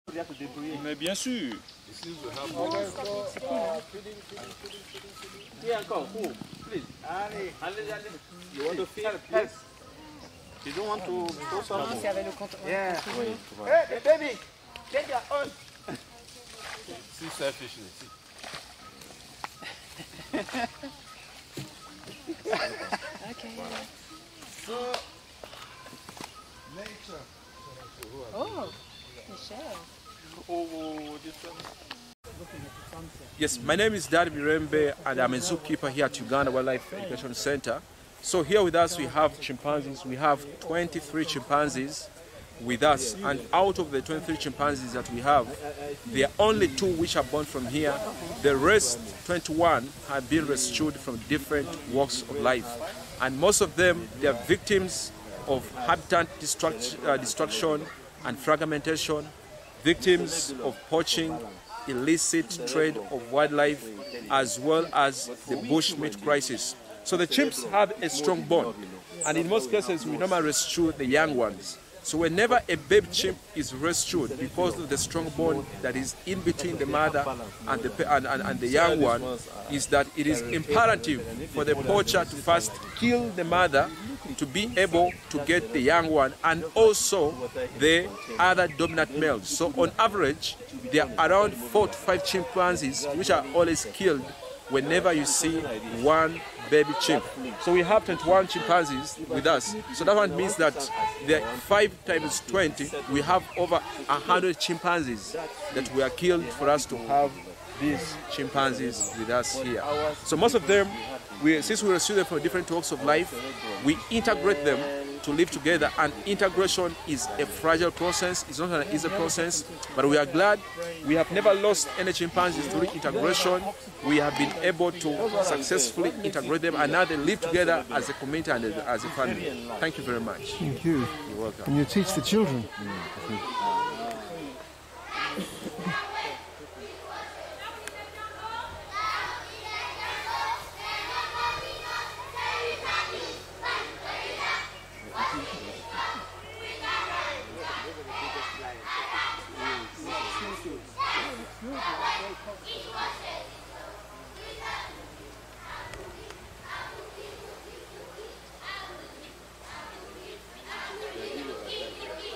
Chimpanzee tool-use: using branch to get fruit in moat at Uganda Wildlife Education Centre, Entebbe.
July 14th is WorldChimpanzeeDay - a day to celebrate our zoological next of kin, with whom we share some 99 per cent of our DNA. They are self-aware, social beings whose communities across 21 African countries are under threat. In this video, we see a rescued chimpanzee in the Uganda Wildlife Education Centre demonstrating inventive tool-use to retrieve a piece of fruit from the moat by fetching a branch and skilfully drawing it within reach. Keeper Dan Mirembe then explains that when poachers capture baby apes for the illegalwildifetrade, their mother and other family members are killed, so 20 rescued chimps likely represent 100 dead members of this endangered species. This is why the education work done by primate sanctuaries across Africa is so important, so the next generation understands the importance of conservation. Not only do chimpanzees deserve our respect for ethical reasons, their habitat is critical in global efforts to prevent dangerous climatechange. Chimpanzees are among the most important seed-dispersal agents - GardenersoftheForest so protecting them today ensures there are trees to sequester and store carbon tomorrow.
Many areas of chimpanzee habitat straddle international borders and as the apes move back and forth according to seasonal food sources, cooperation between governments is essential.  This is why the species is protected under the UN Convention on Migratory Species and why efforts are under way to protect them and their cultural diversity, e.g. using the recently published IUCN Action Plan